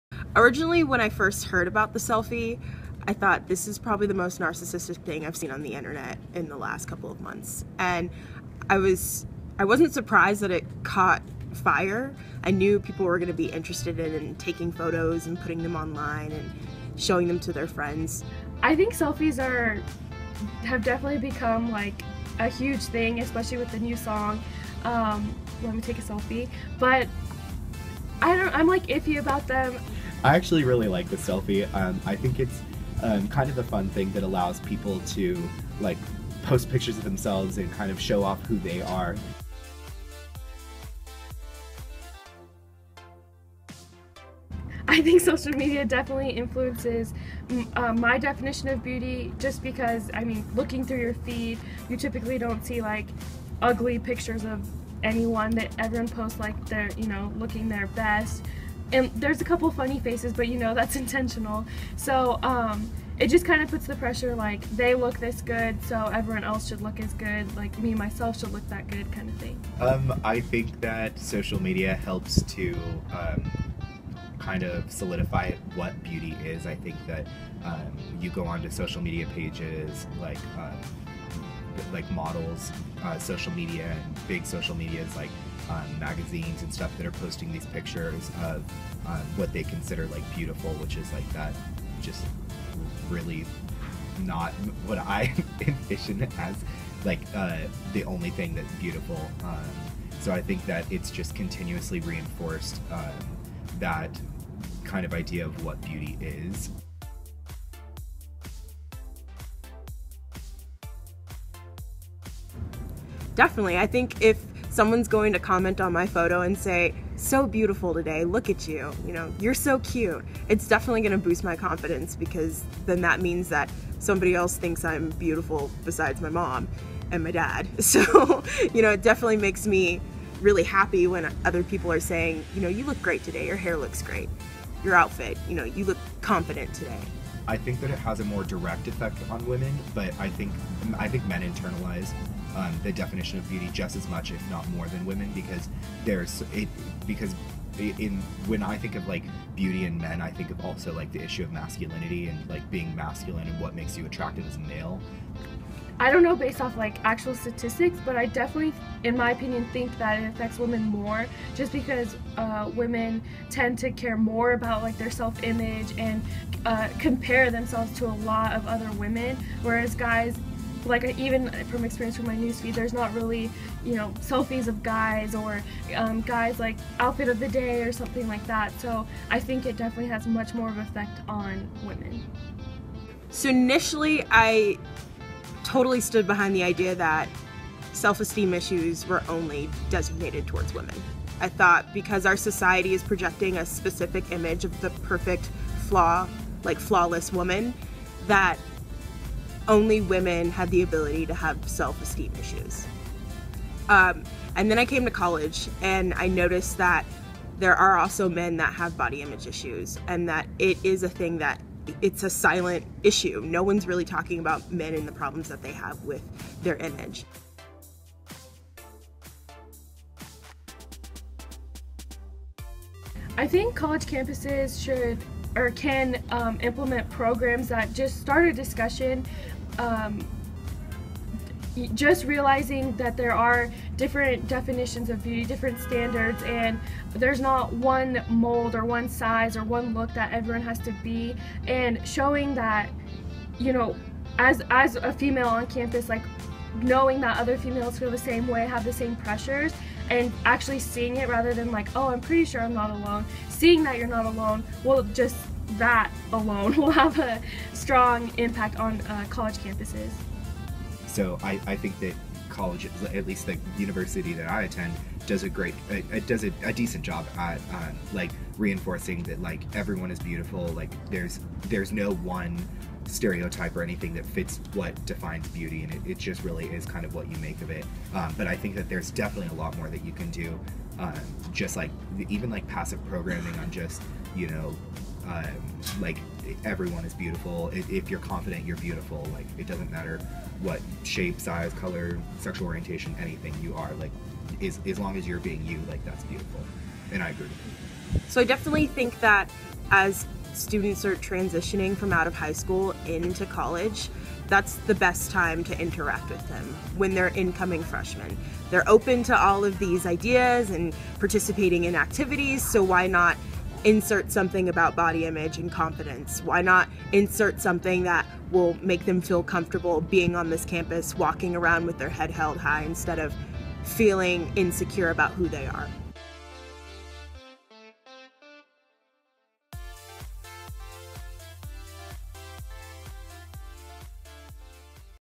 Redefine Beauty
How does socialmedia influence how students view bodyimage? How do students view body image and selfconfidence? What can college and university campuses do to help improve body image and self confidence? Does social media affect men and women's body image equally? What do you think of the selfie?